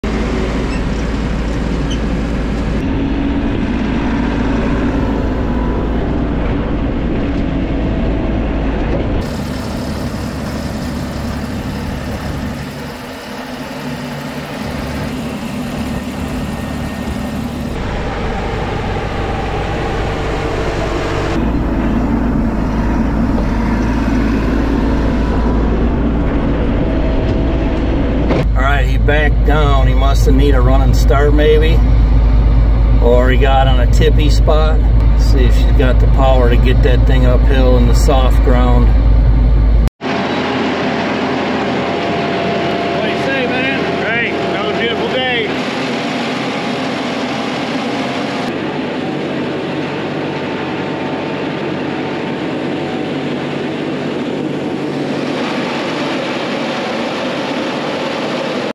Real life DIY land improvement projects!
Join us for a day on a horse ranch with stunning horse pastures! Explore the beauty of the Southern Illinois land and learn about improving horse pasture management.
A totally different kind of today! Today I helped out one of our neighbor's on our rural Southern Illinois rural road. They have a horse farm and today we worked on some horse pastures, moving a lot of dirt, repairing water erosion runoff and installing a new culvert pipe for water erosion and guiding the water runoff into their pond. Thanks for stopping by!